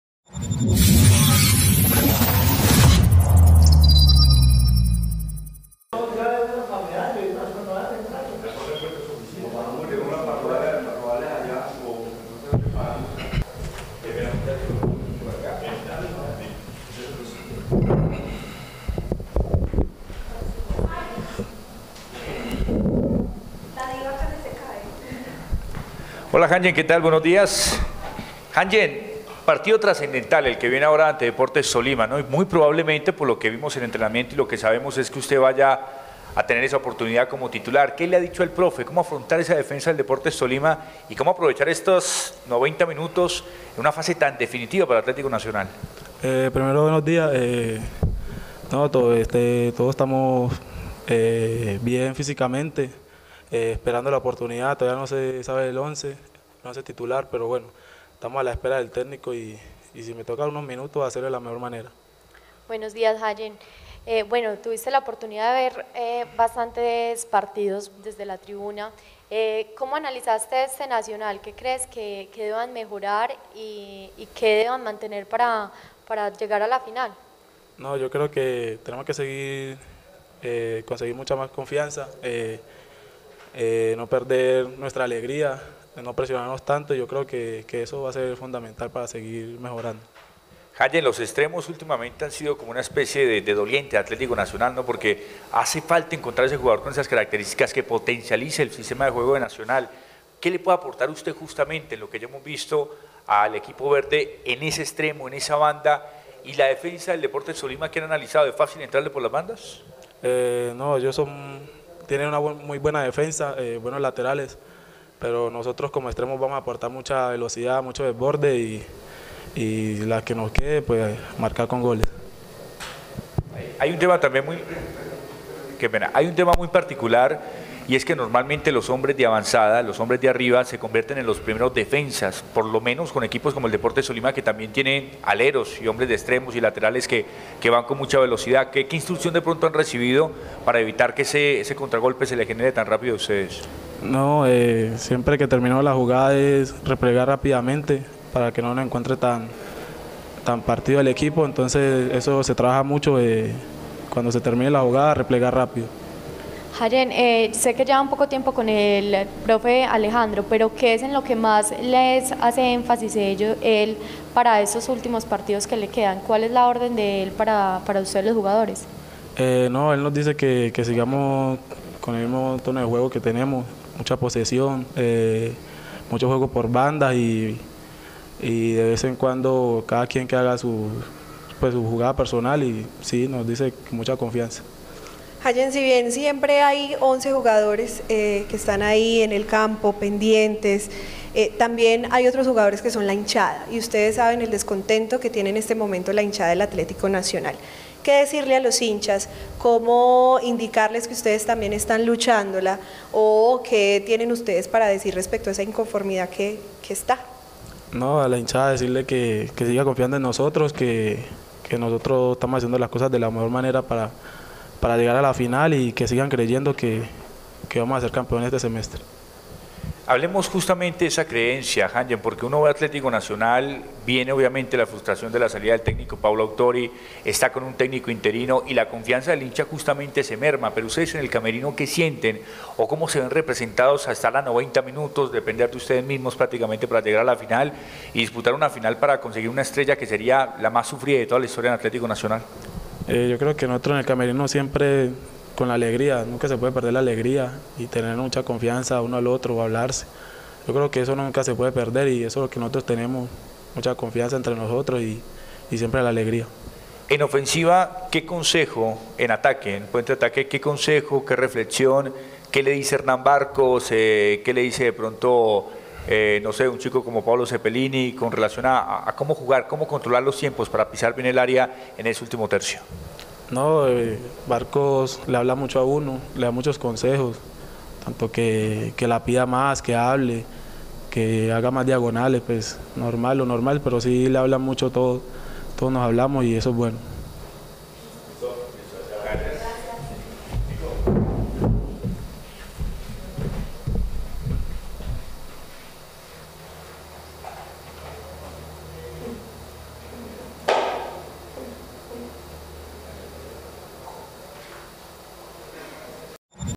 Hayen Palacios:"Sigan confiando y creyendo en nosotros"...
El delantero de Atlético Nacional, Hayen Palacios, envió un mensaje de positivismo a toda la hinchada verdolaga:"Sigan  confiando y creyendo en nosotros"...

Con mucho optimismo, Hayen cree que Nacional va a llegar a la final del fútbol colombiano.

Con ustedes Hayen Palacios...Veamos...